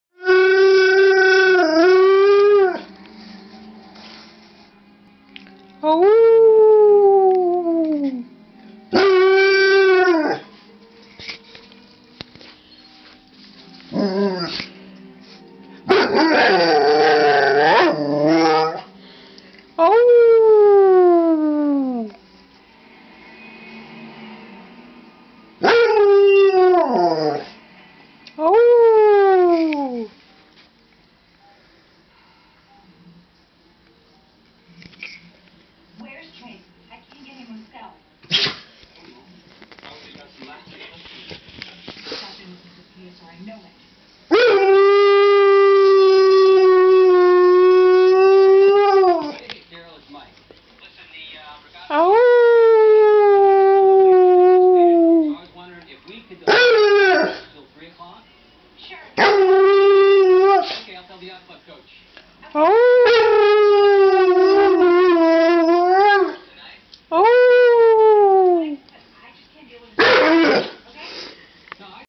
Bear talking
excuse my awful attempt to howl, but here I am talking to Bear. I was bored.